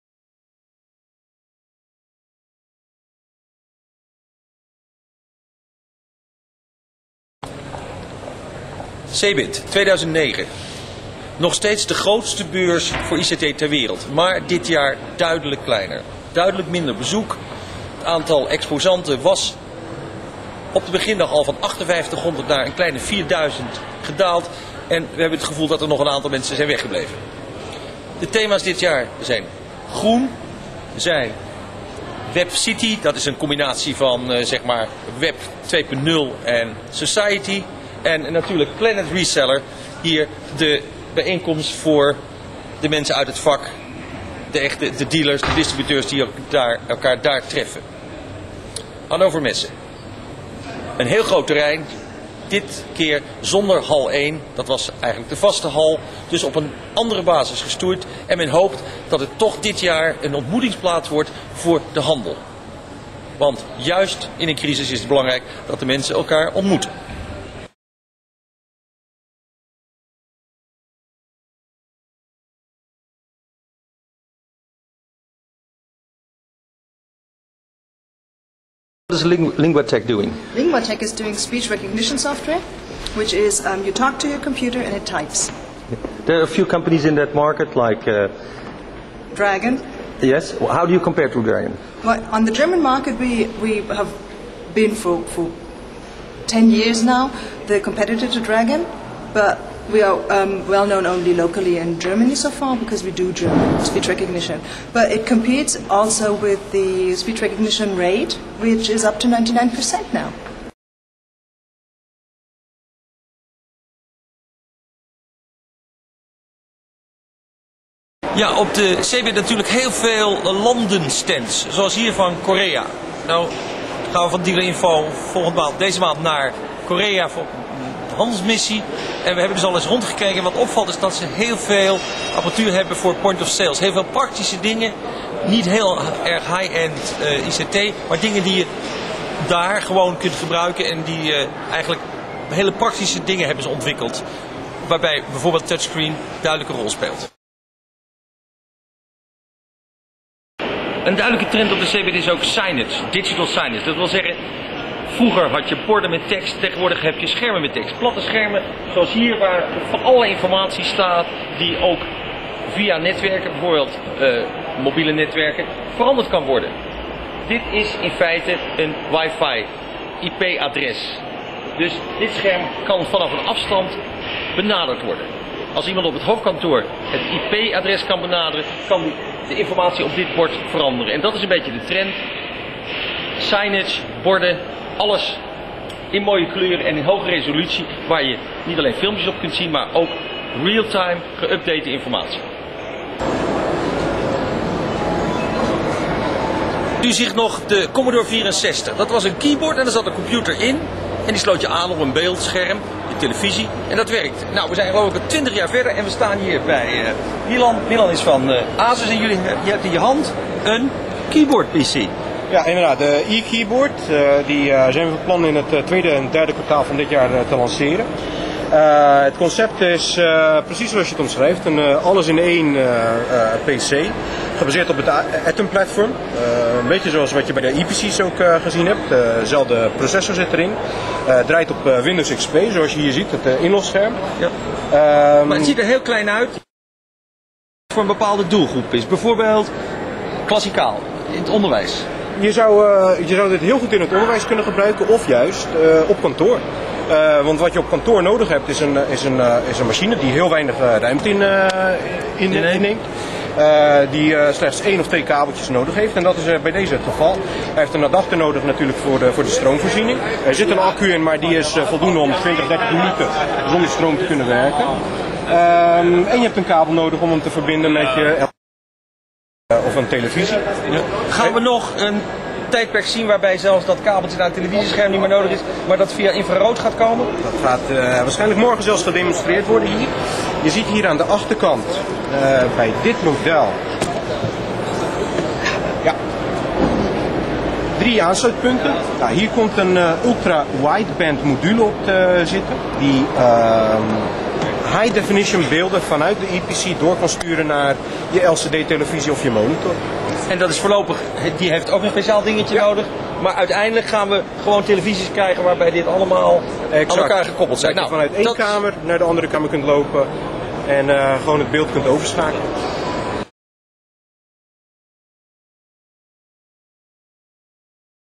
comments from Cebit 2009 Hannover In Dutch)
Reportage vanaf de CeBIT door Luc Sala van Dealer-Info.